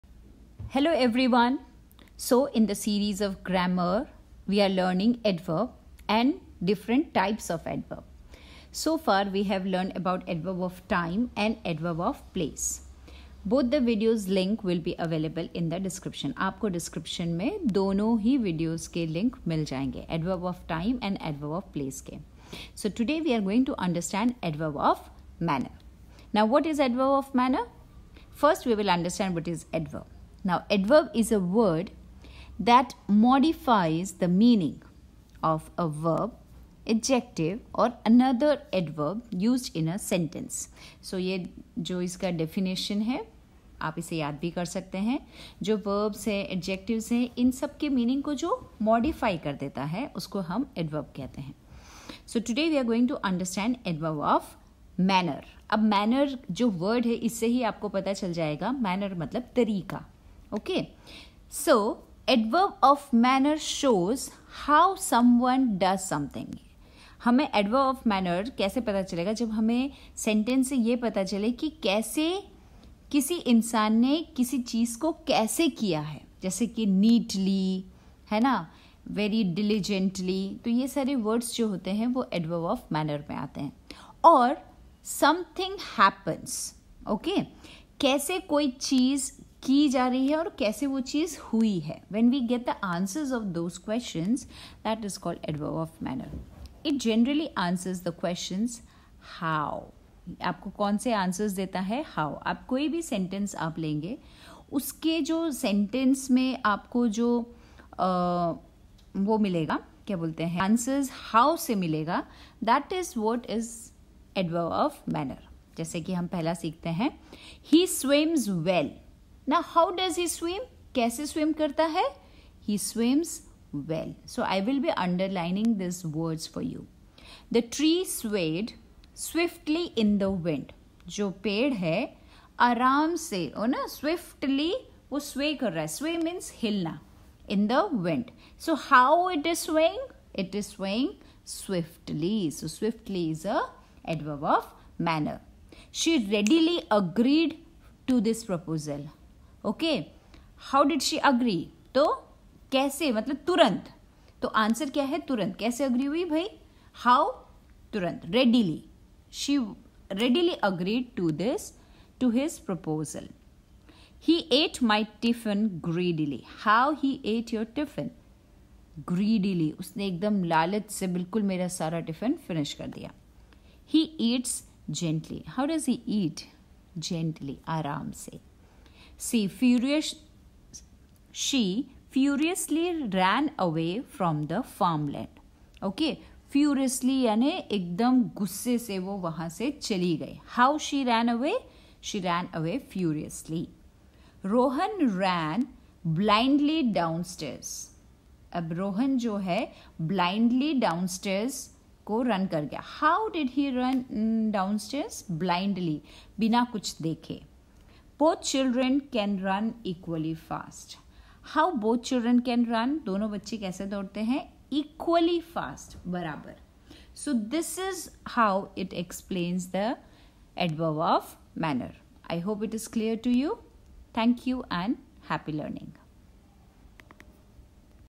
adverb | adverb of manner| grammar| kvs| Easyenglish | ncert
Adverb of manner
Here in this video you will find the easy explanation  of adverb of manner with examples and the link of adverb of time and place is also available.